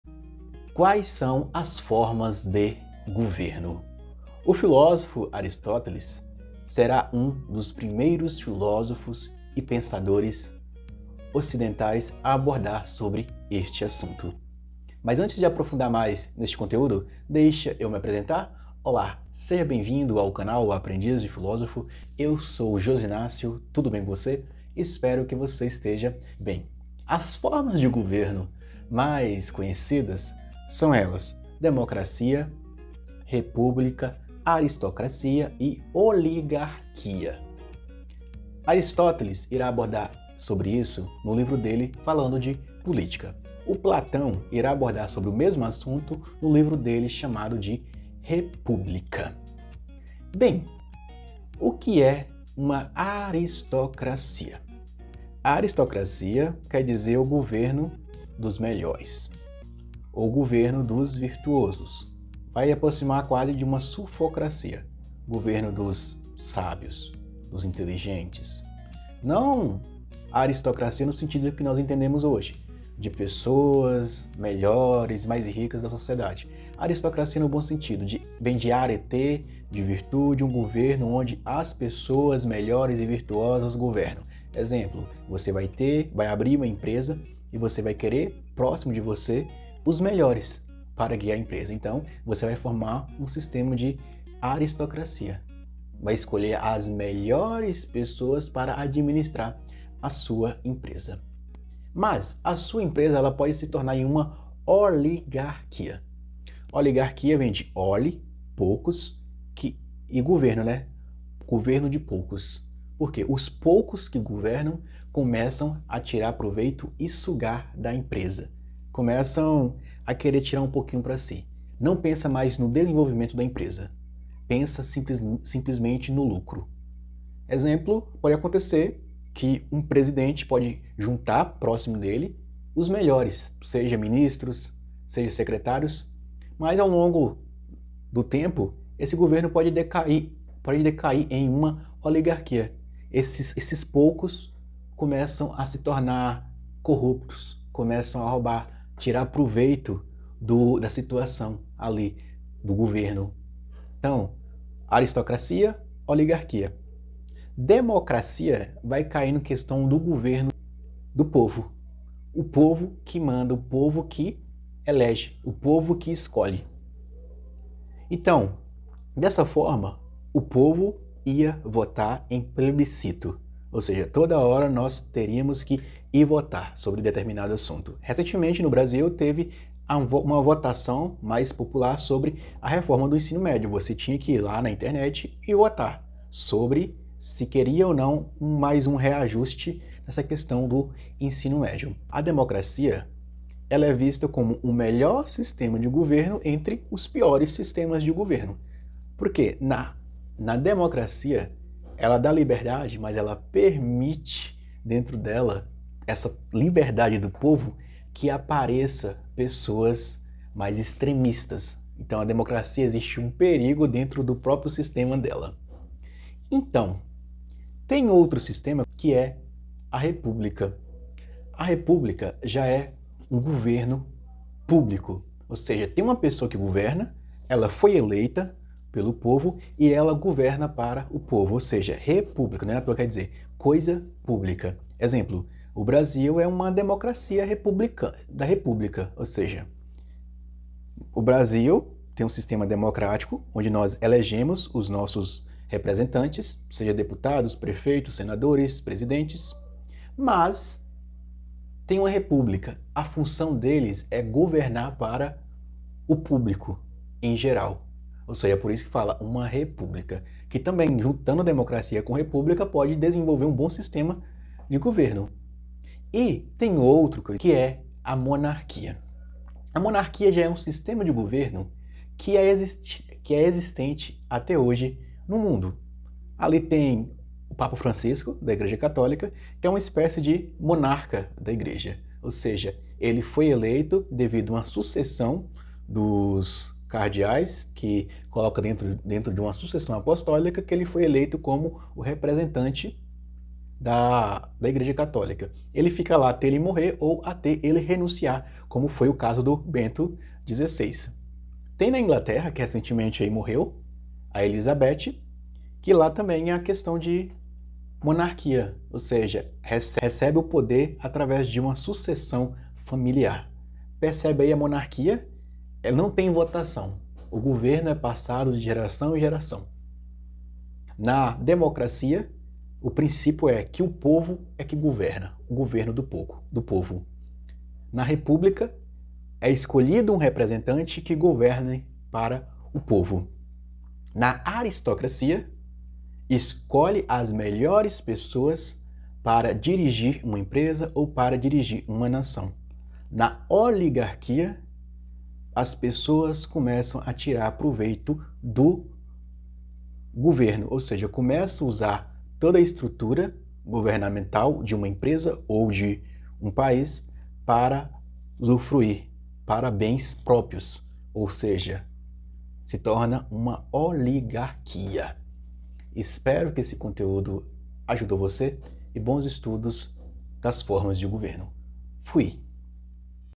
Vamos pensar as formas de governo para o Brasil de hoje, e entender como cada uma funciona
Neste vídeo, vamos explorar as diferentes formas de governo, trazendo as ideias dos filósofos Aristóteles e Platão, e fazendo uma análise da atual situação política do Brasil.

Aristóteles, em sua obra "Política", classificou as formas de governo em monarquia, aristocracia e democracia, alertando para os perigos que cada uma delas pode enfrentar. Já Platão, em "A República", apresentou sua visão de um governo ideal, com um filósofo-rei no comando.

No contexto atual do Brasil, que adota o sistema democrático, enfrentamos desafios como a corrupção, desigualdade social e falta de representatividade política. A corrupção mina a confiança nas instituições, a desigualdade social afeta a igualdade de direitos e oportunidades, e a falta de representatividade pode enfraquecer a democracia.

É fundamental que o Brasil busque aprimorar seu sistema democrático, combatendo a corrupção, reduzindo as desigualdades sociais e promovendo a participação popular. Assim, poderemos construir uma sociedade mais justa e verdadeiramente democrática.

Assista a este vídeo para entender melhor as formas de governo, refletir sobre as ideias de Aristóteles e Platão, e refletir sobre a atual situação política do Brasil.

Veja mais conteúdo filosófico no canal O Aprendiz De FILÓSOFO.
Qual a Relação De Corpo e Alma? (Filosofia Da Mente a Dualidade Entre Corpo e Alma)